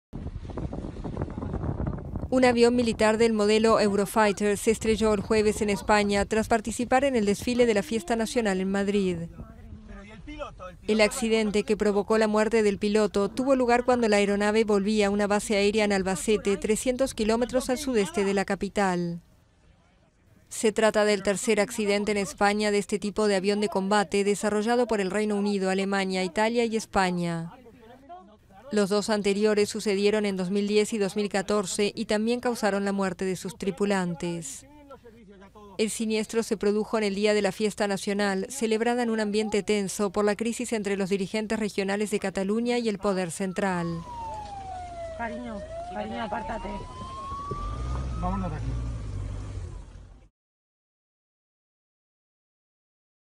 Avion militar se estrella en España tras la fiesta nacional
Mas noticias en diario El Articulista...